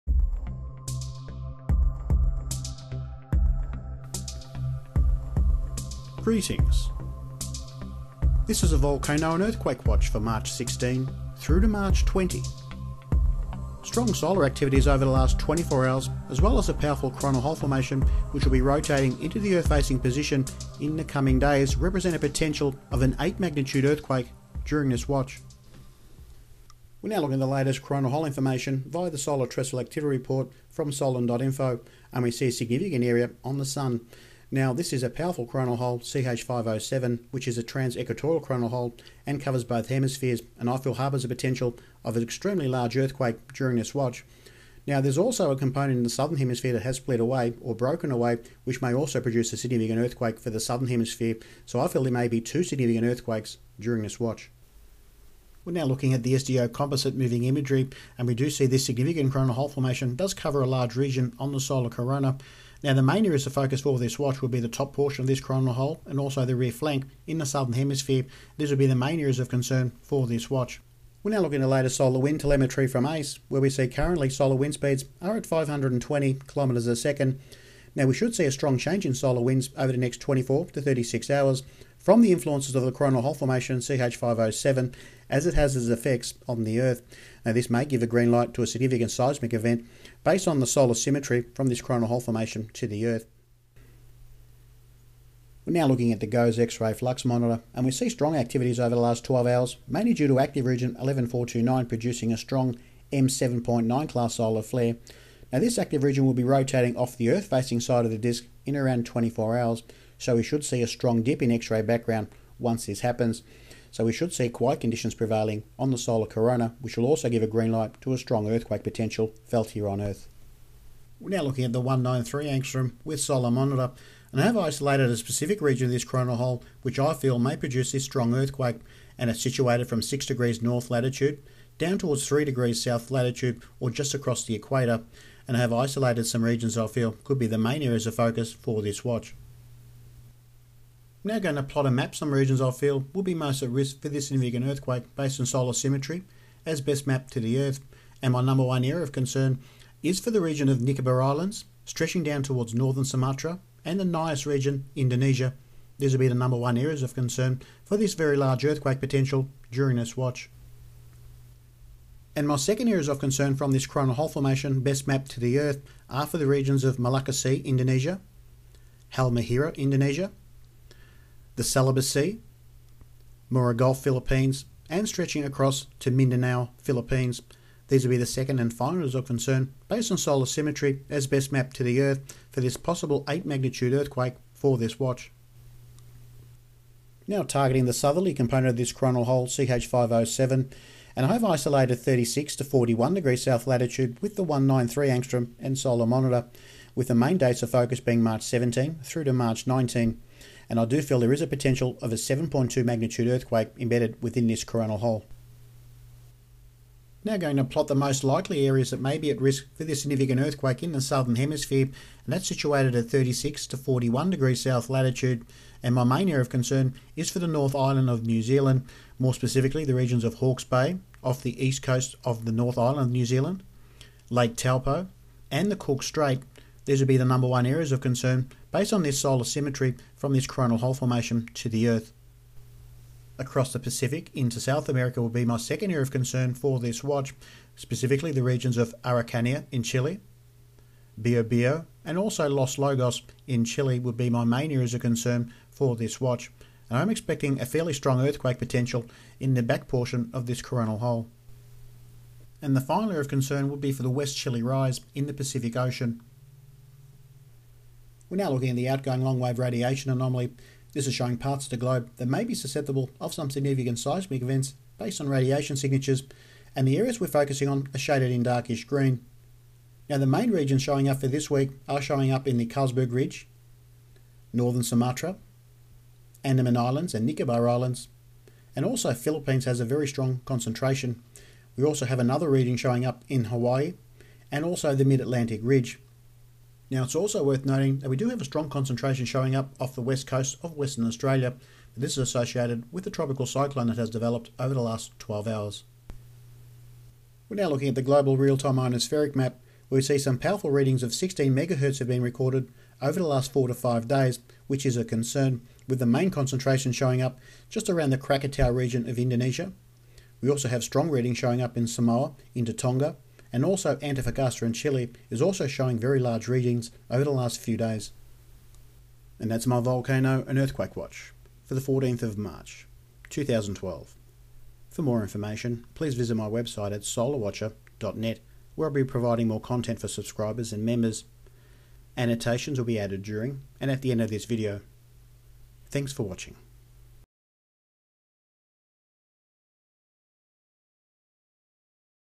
Volcano / Earthquake Watch March 16-20, 2012
Targeting Coronal Hole (CH507) which covers a large region across the solar corona. After analysis i have isolated (3°S - 6°N Latitude) for a potential 8.0 Magnitude Earthquake.
Areas that could be in line for this event are: Nicobar Islands- India Region, Northern Sumatra-Indonesia, Nias Region- Indonesia, Moro Gulf/Mindanao Philippines, Celebes Sea or the Molucca Sea. Time frame March 16-18.

Targeting the southern extension of (CH507), and isolating the 36-41°S latitudes as the most likely area that could receive a 7.2 Magnitude Earthquake during this watch. Time frame March 17-19. Possible areas that could be affected are:
North Island New Zealand, Bio-Bio Chile, Los Lagos Chile or the West Chile Rise.

OLR Anomalies are this week are Carlsberg Ridge, Northern Sumatra, Nicobar Islands, Hawaii, Mid-Atlantic Ridge and the Philippines

Ionospheric anomalies continuing to show high readings (16Mhz) in the following areas: Antofagasta Chile, Samoa, Tonga, Northern Sumatra and the Banda Sea

Updates and annotations will be added to the youtube video description box.

The spinning world globe at the start of the video is a free program called Earthquake3D
The use of images and voice from this video may NOT be used by another party and this video will not mirrored under any circumstances as annotations will be added to the video with important updates.

**updates and annotations for mobile phone users**
Sharp rise in solar winds (800km/s) has delayed the forecast slightly march 16th, earthquakes should begin once solar winds drop under 450km/s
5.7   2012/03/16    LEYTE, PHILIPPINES
Solar Winds drop under 450km/s March 19th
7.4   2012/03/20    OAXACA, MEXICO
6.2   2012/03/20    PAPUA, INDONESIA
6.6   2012/03/21   NEW GUINEA, PAPUA NEW GUINEA